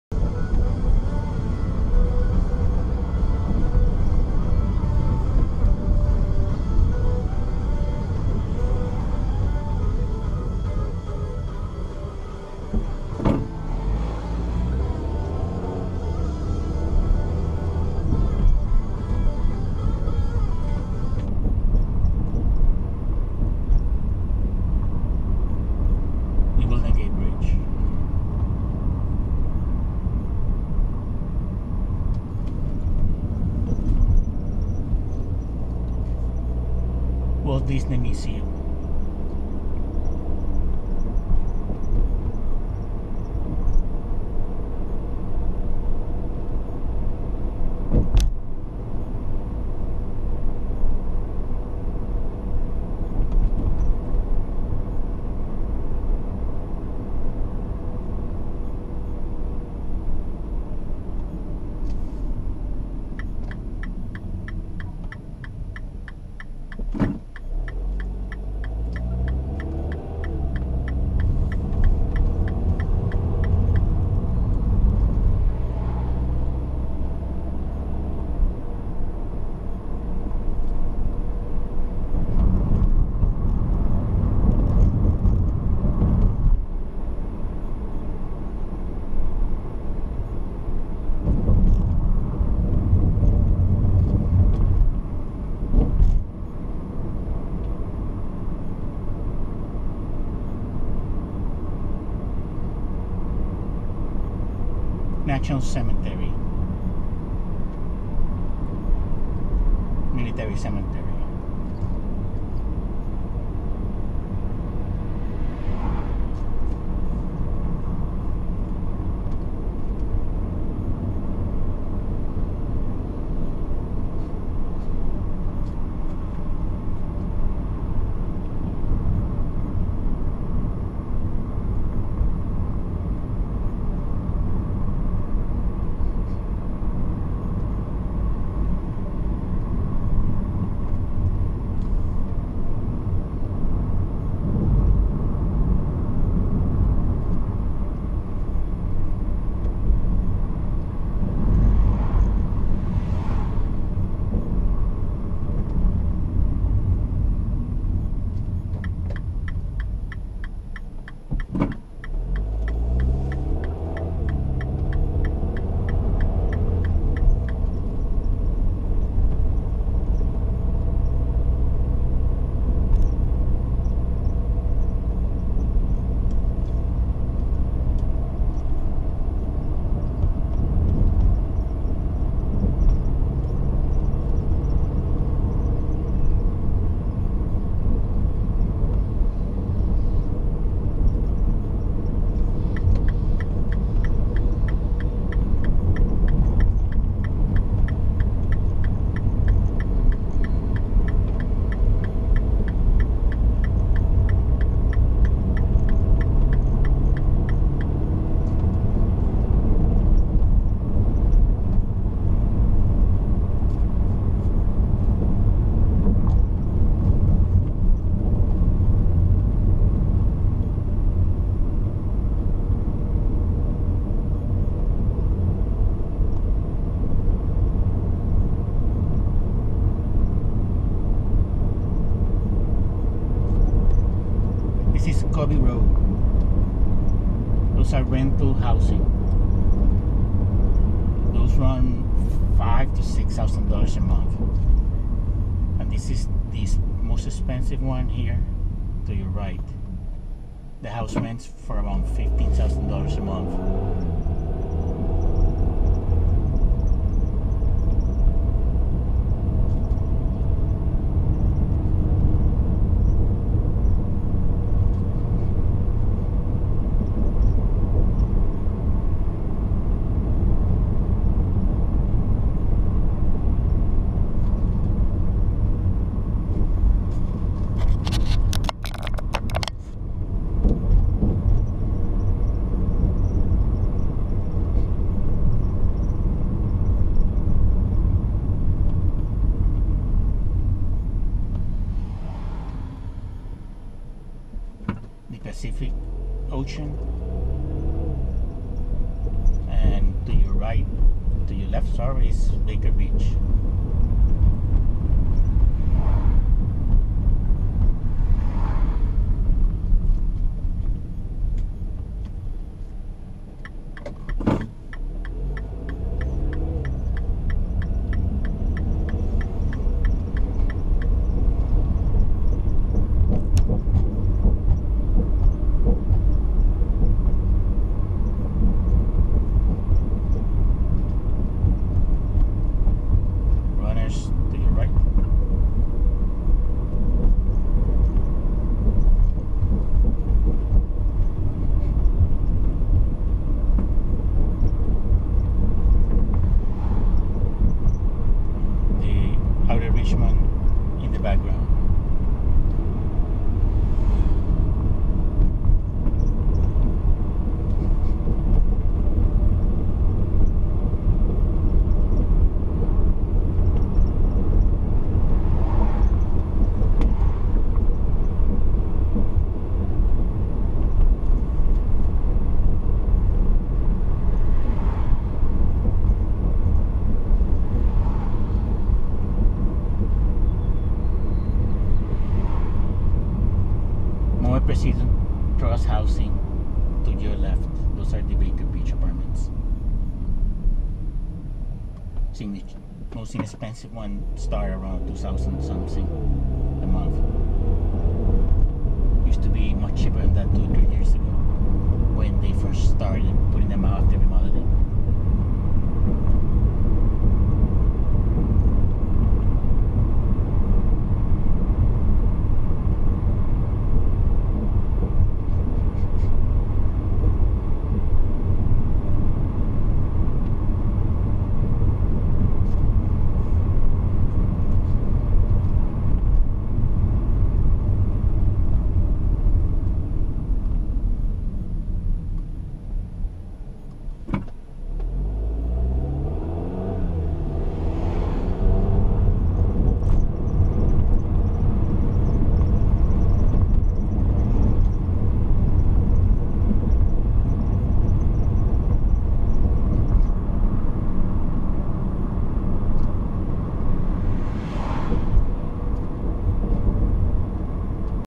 Driving through the Presidio in SF
Driving from the Presidio YMCA to Baker Beach. You can see the Golden Gate Bridge in the distance and the Pacific Ocean.